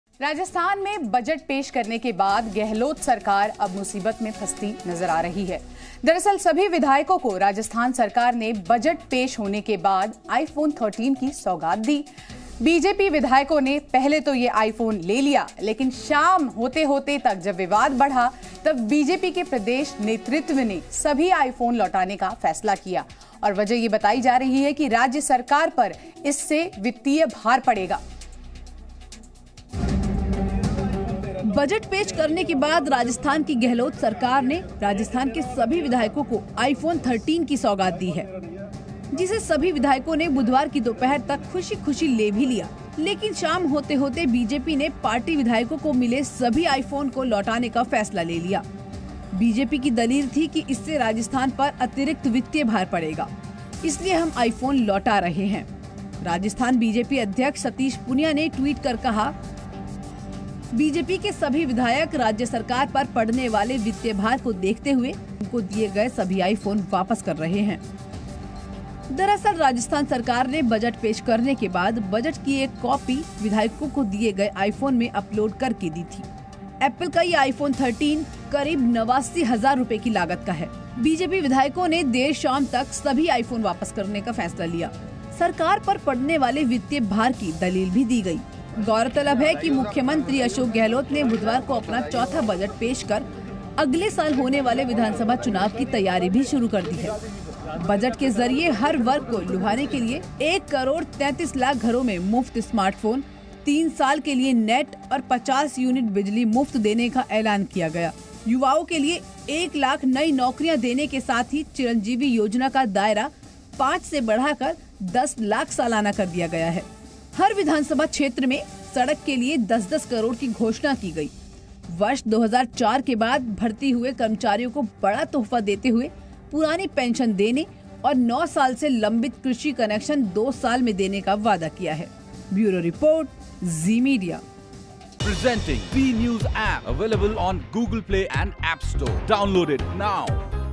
Rajasthan: BJP के 71 विधायक गहलोत सरकार को लौटाएंगे iPhone | Latest Hindi Update | Top News Today
भारतीय जनता पार्टी ने राजस्थान विधानसभा में बजटपेश होने के बाद सरकार की तरफ से मिले आईफोन लौटाने का फैसला किया है. राज्य सरकार पर पड़ने वाले वित्तीय बोझ को ध्यान में रखते हुए यह फैसला किया गया है.

BJP has decided to return the iPhone received by the government after the presentation of the budget in the Rajasthan Legislative Assembly. Keeping in mind the financial burden on the state government, this decision has been taken.